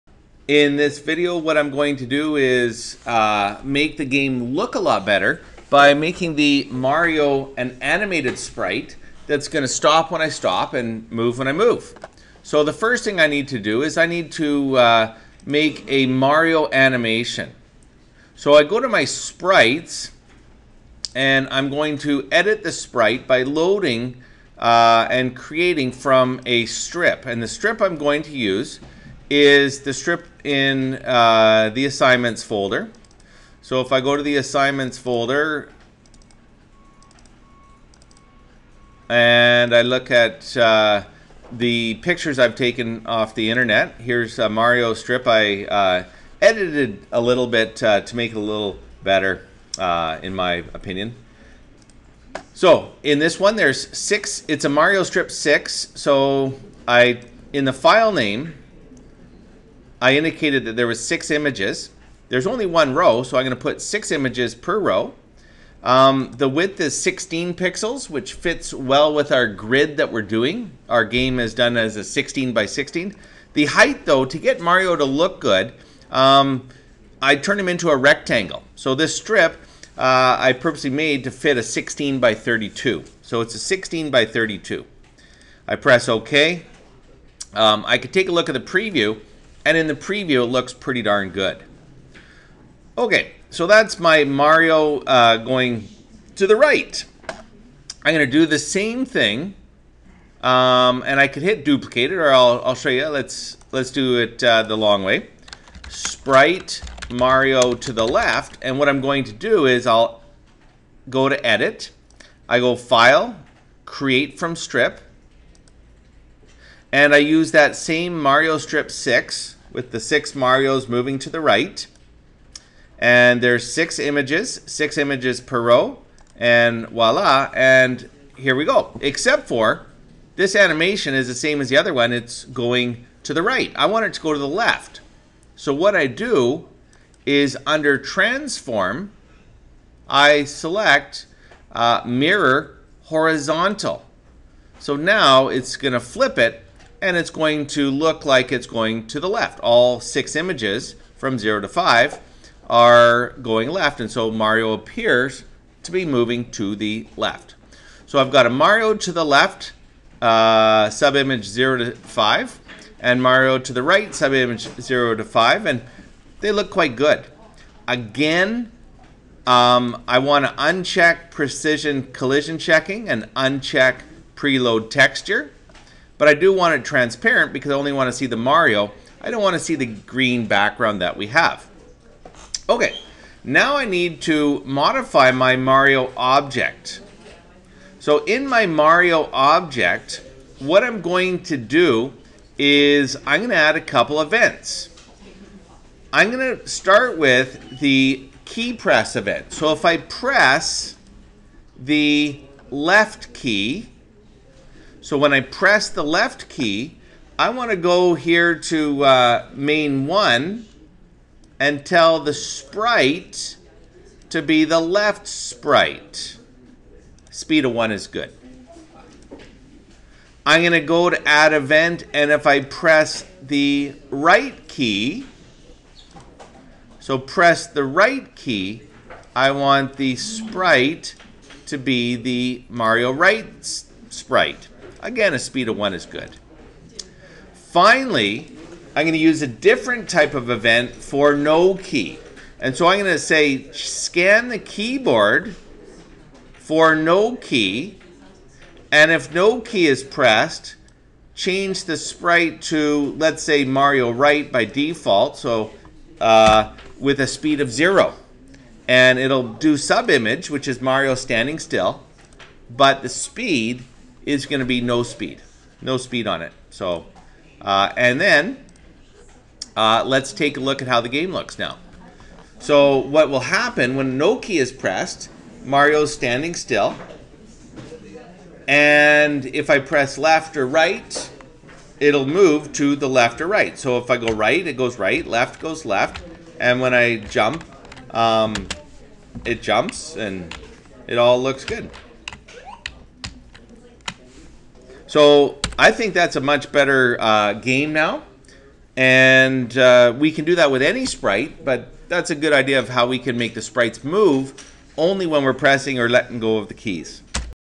Mario05
Using GameMaker 7 to create an animated sprite from a strip.  As well we will change the sprite based on what key is pressed.  If we press the right key, the right walking sprite is displayed. Left, the left is displayed.  In the keyboard NO KEY even, we display the animated sprite with a speed of 0 so it looks like we have stopped.